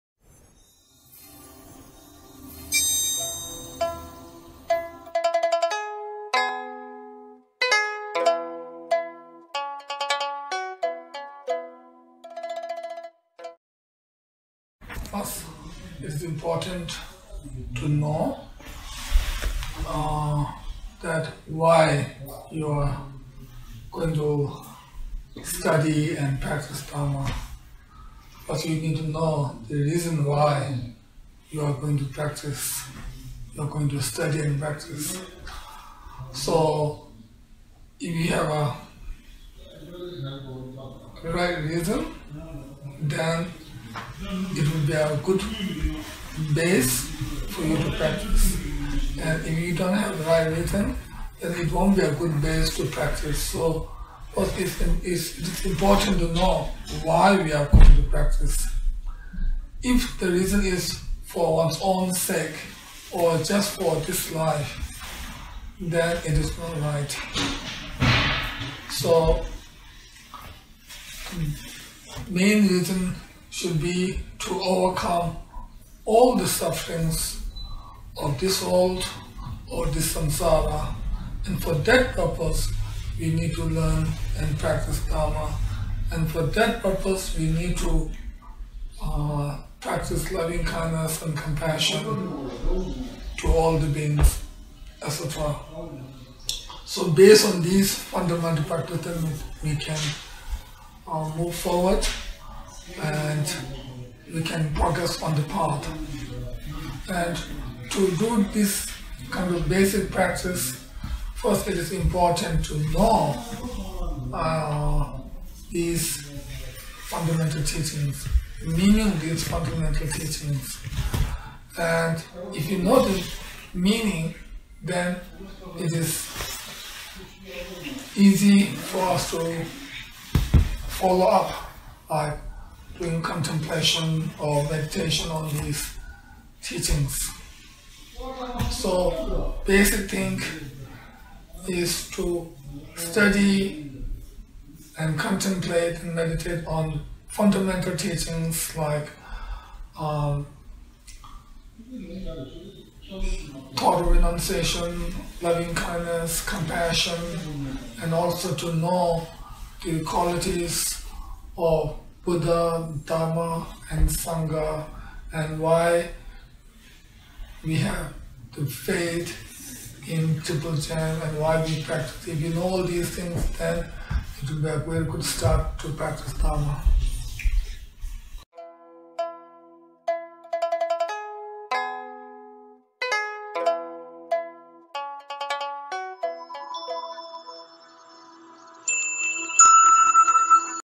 How should I study and practice the Dharma? (Tibetan subtitles)
An advice given by H.H. the 42nd Sakya Trizin, Ratna Vajra Rinpoche.

I’m new to Buddhism. How should I go about studying and practicing the Dharma?
དྲི་བ།  ང་རང་ནང་ཆོས་ལ་གསར་དུ་བཞུགས་པ་ཞིག་ཡིན། ངས་ཇི་ལྟར་བྱས་ནས་ནང་ཆོས་ལ་སློབ་སྦྱོང་དང་ལག་ལེན་བྱེད་དགོས།